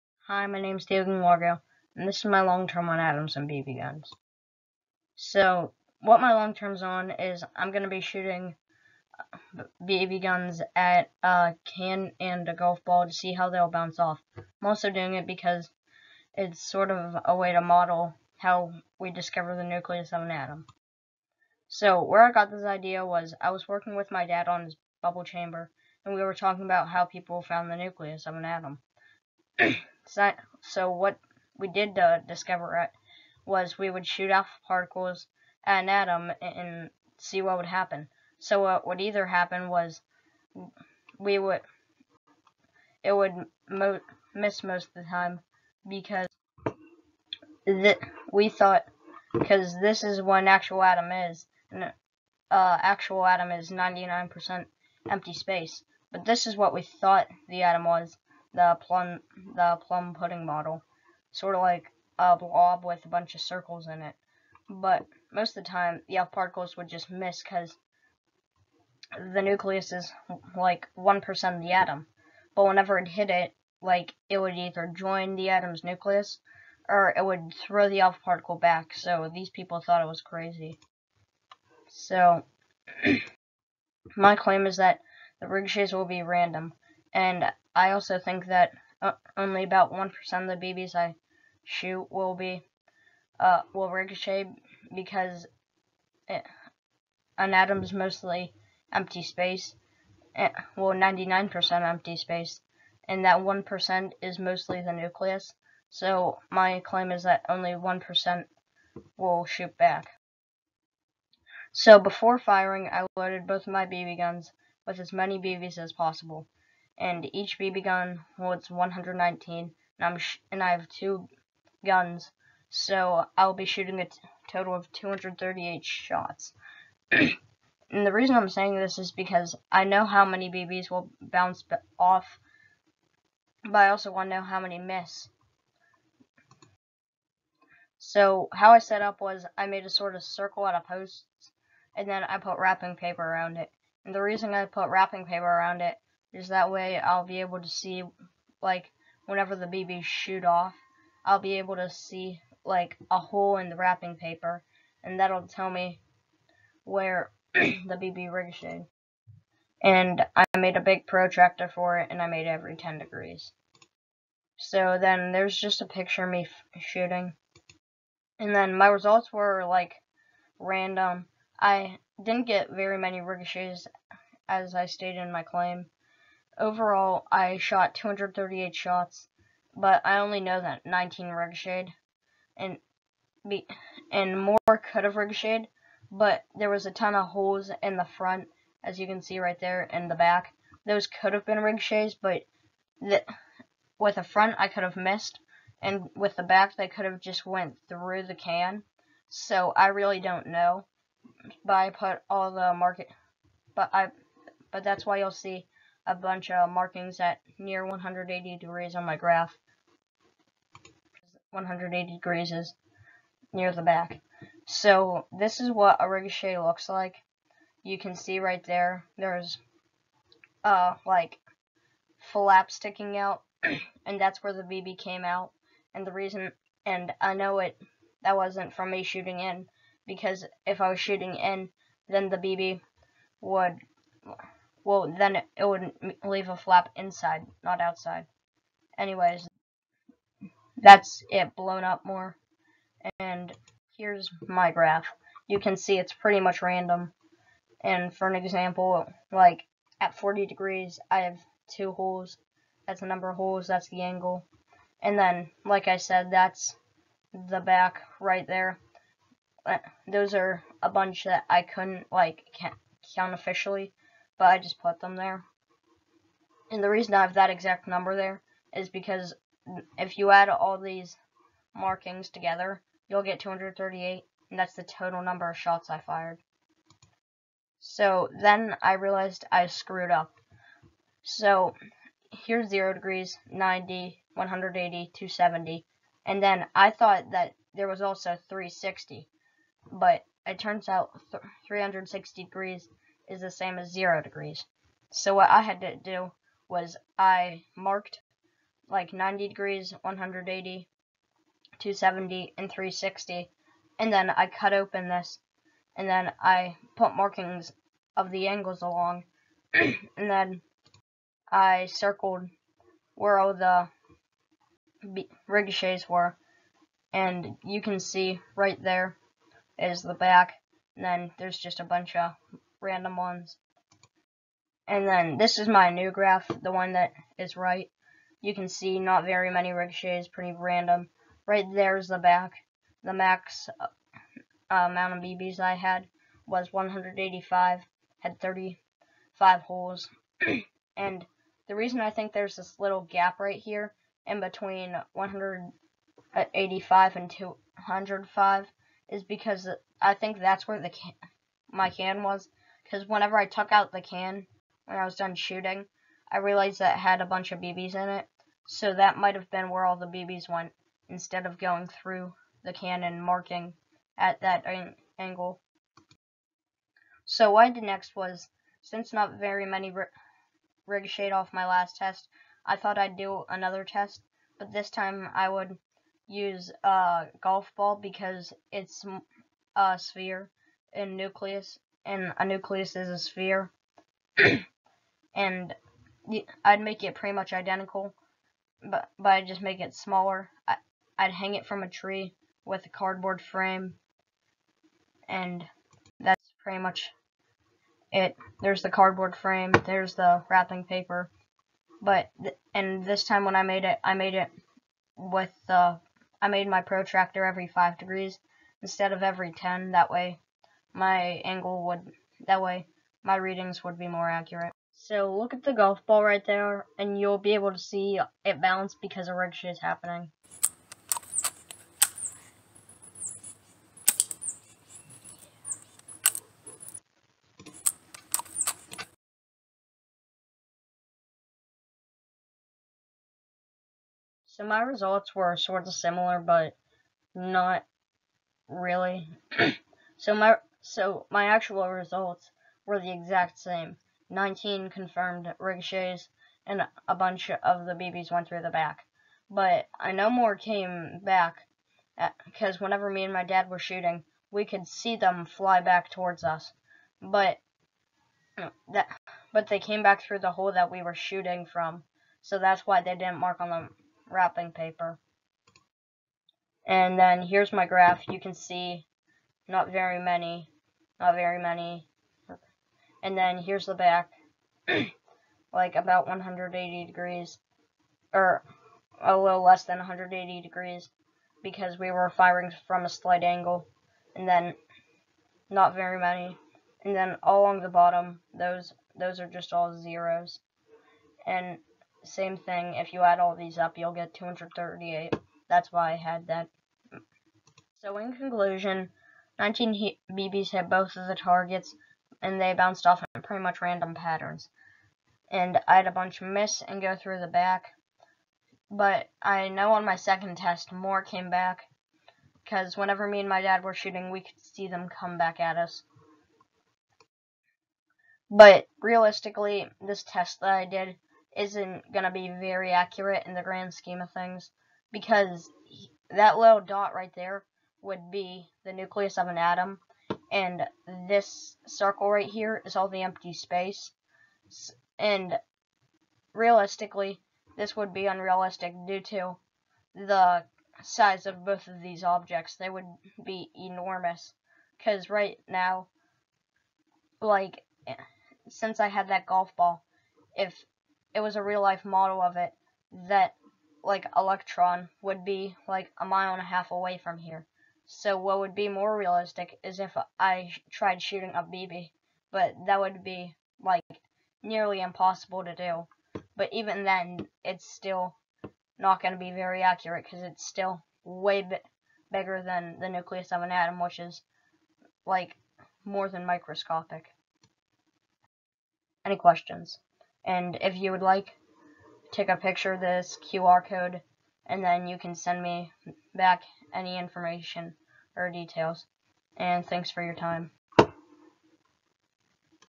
THEO GING-WARGO – Modeling the Nucleus with a BB Gun - 5th Grade Science
THEO GING-WARGO – Modeling the Nucleus with a BB Gun - 5th Grade Science - Presented at the Fifth Annual Freedom Area High School Science Conference

This long-term models how we found the nucleus by shooting alpha particles at an atom. I got this idea when I was helping my dad build a particle accelerator. I am modeling that by using bbs shot from a bb gun as alpha particles and a golf ball as a nucleus. Wrapping-paper served as an indicator were the bbs ricocheted.